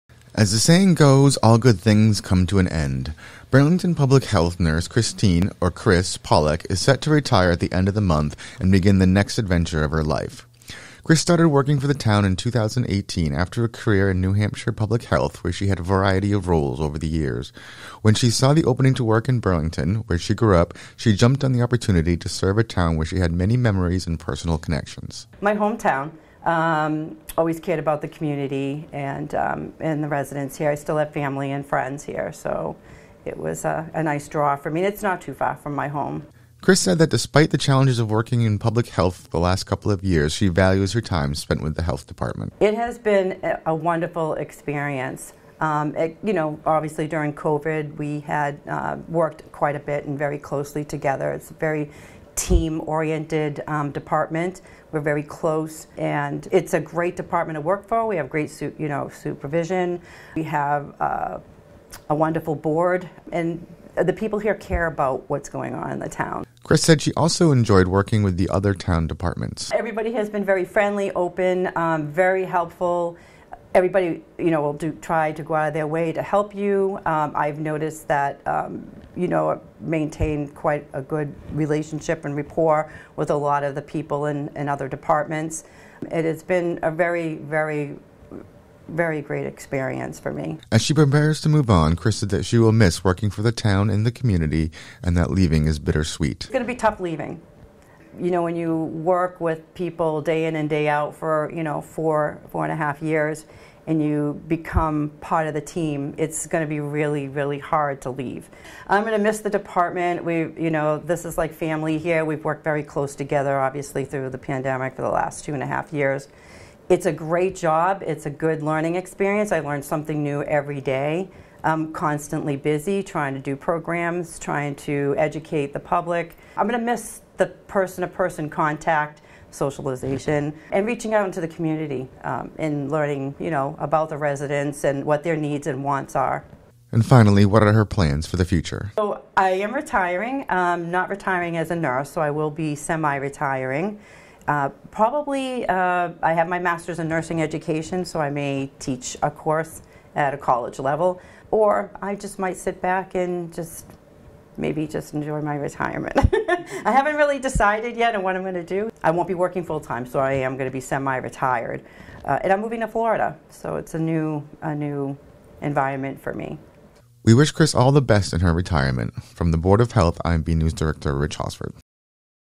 Burlington Public Health Nurse Christine Paulik Retiring
Burlington Public Health Nurse Christine "Chris" Paulik is set to retire from the Public Health Department and move on to her next chapter.